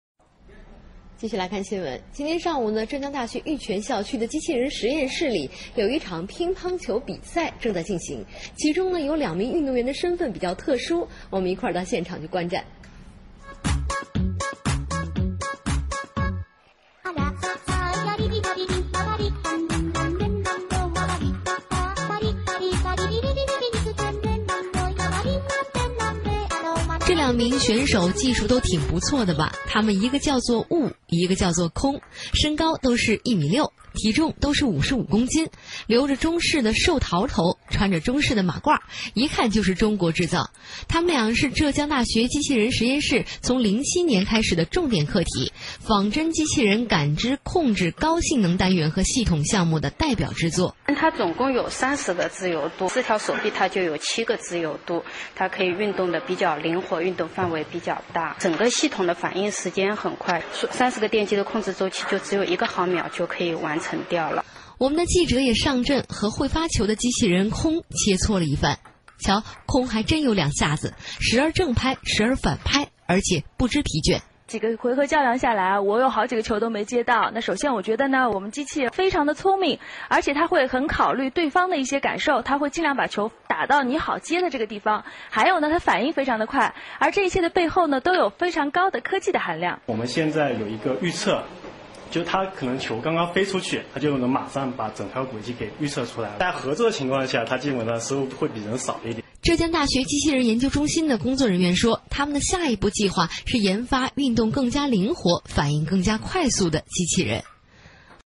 China's Ping-Pong-playing Robots!
Zhejiang University has developed some bipedal humanoid robots and programmed them to play ping-pong!